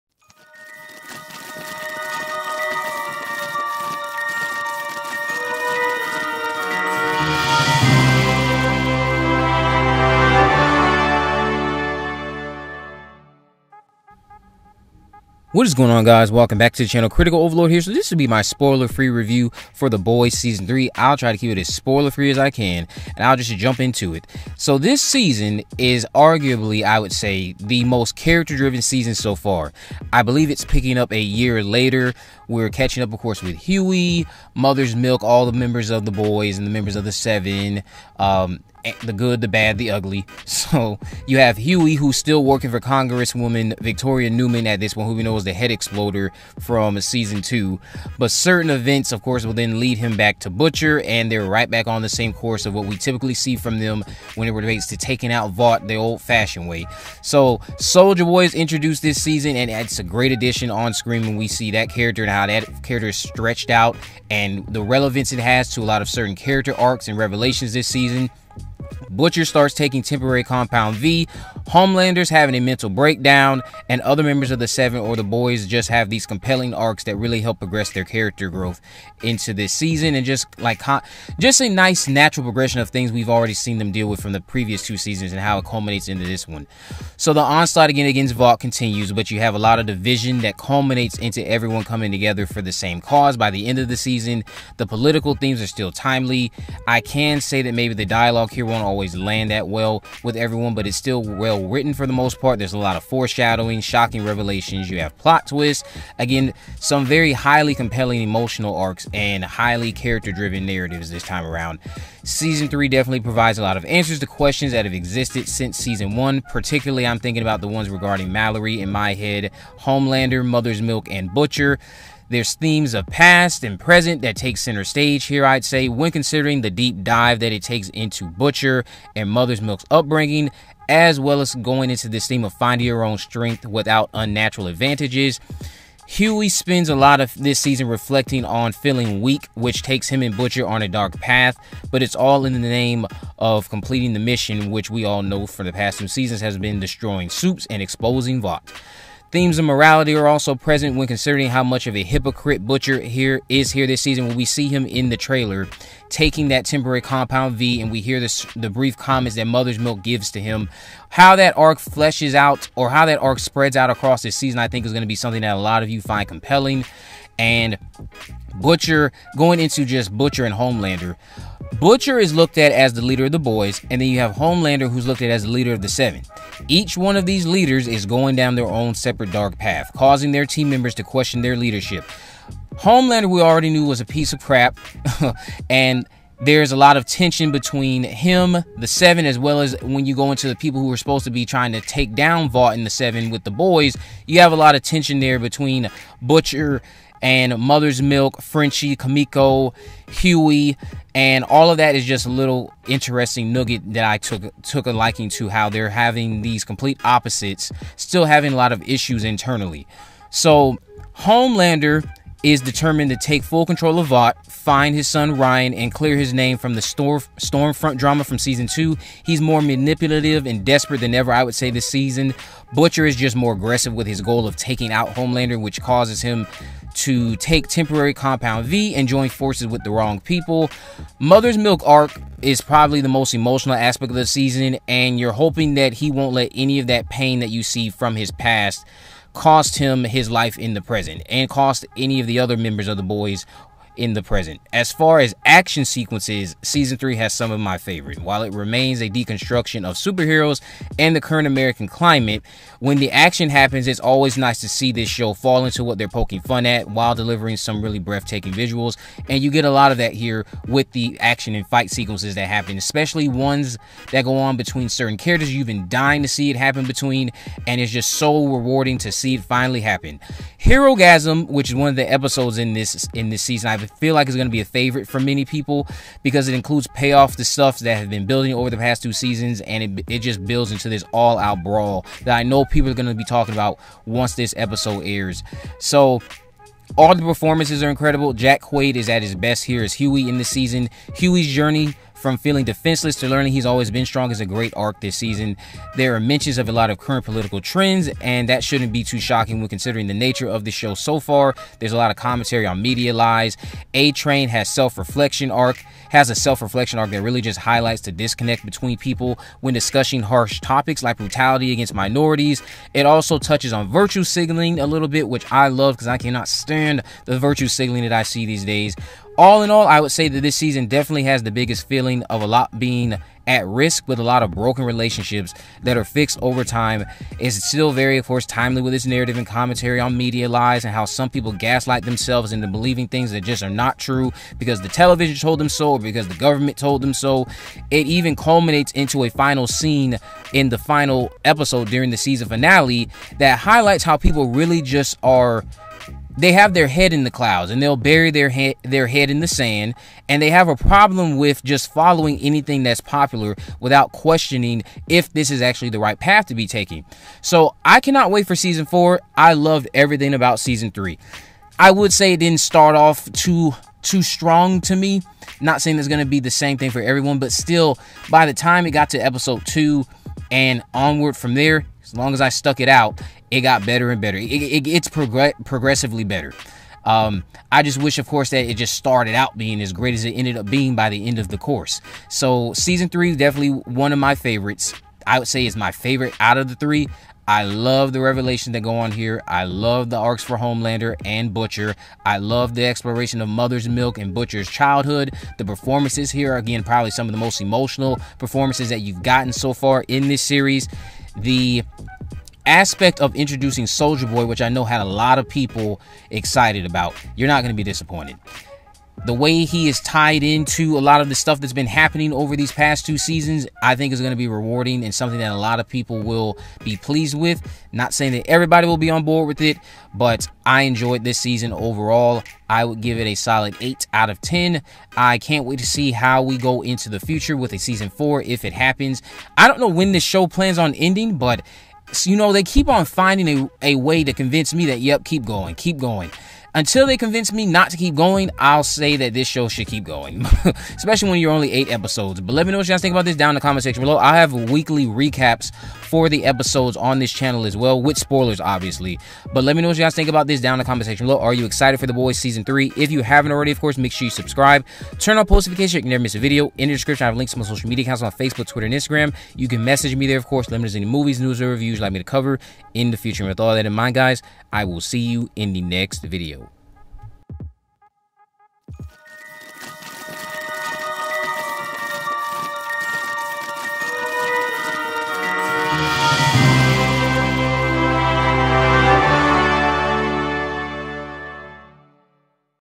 The Boys | Season 3 Review - Spoiler Free
The Boys Season 3 premieres this week on Prime video and I just finished the season. Billy Butcher seems to have taken an if you can't beat em join em approach. Vought is still recovering from the exposure of Compound V, Homelander is insane, and he may come face to face with Butcher in a Supe battle. Jack Quaid from Scream 5 returns as Hughie, a member of The Boys.